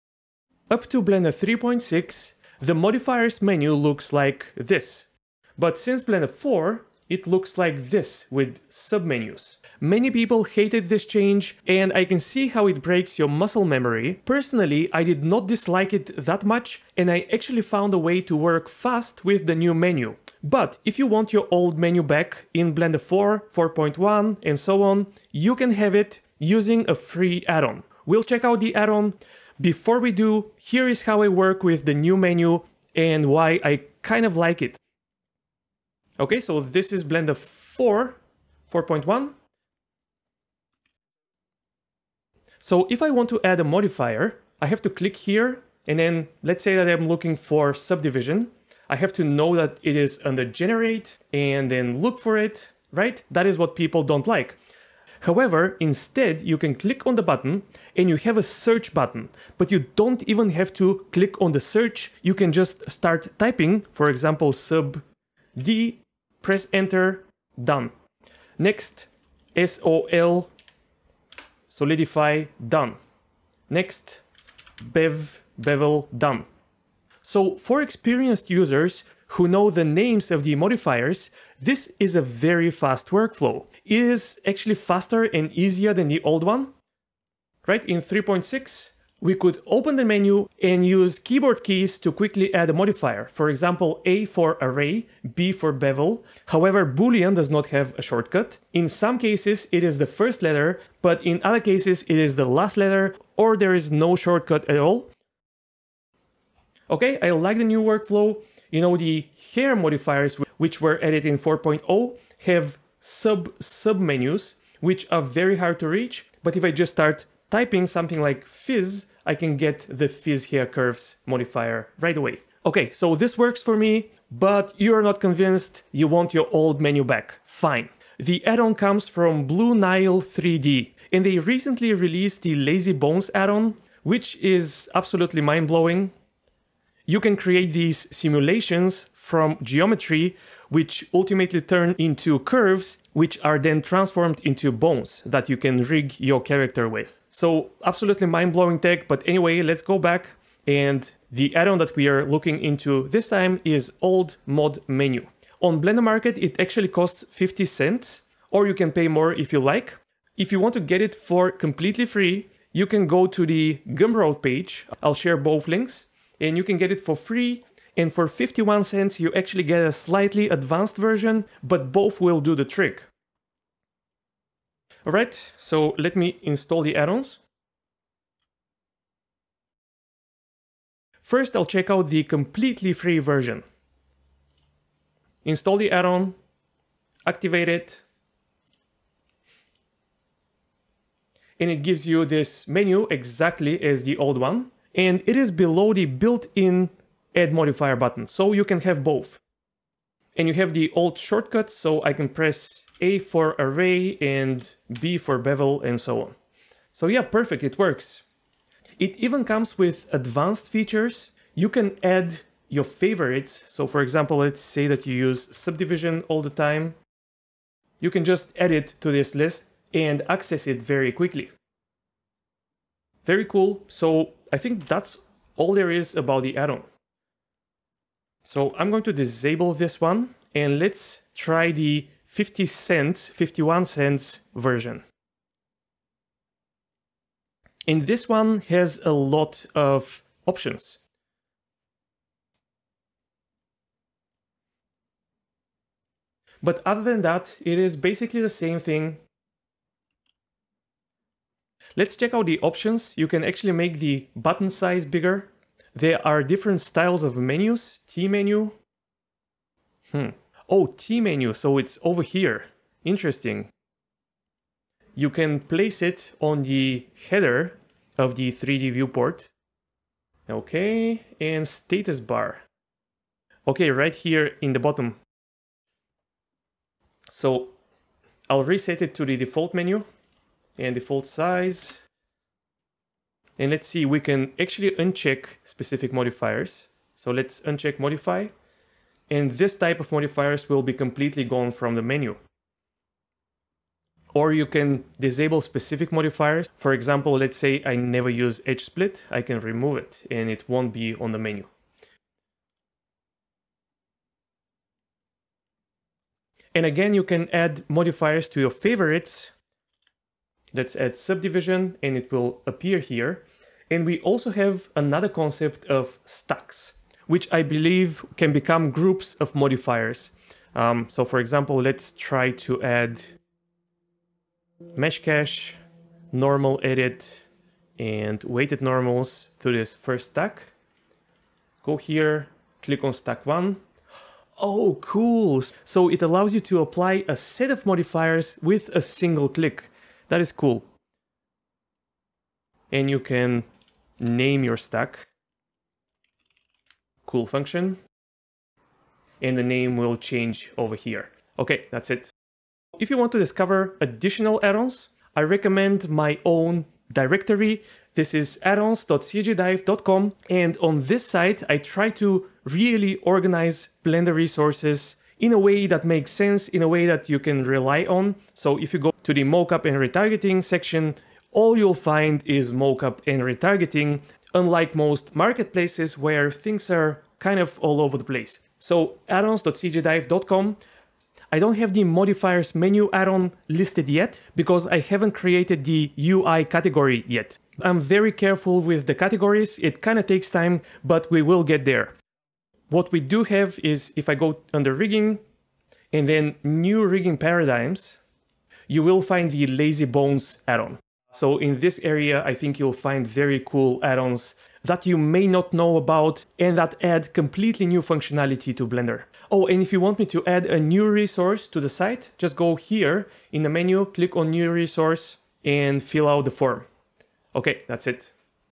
Get the Old Modifiers Menu back in Blender 4 and later!
*Old Menus Addon*

*CHAPTERS*
0:00 Intro Modifiers Menu
0:29 How to use the new Menu
2:02 Get the Old Menu back
2:59 Old Modifers Menus addon 1
3:47 Old Modifers Menus addon 2
5:51 addons.cgdive.com

her32756743edjhfkjhdfslkj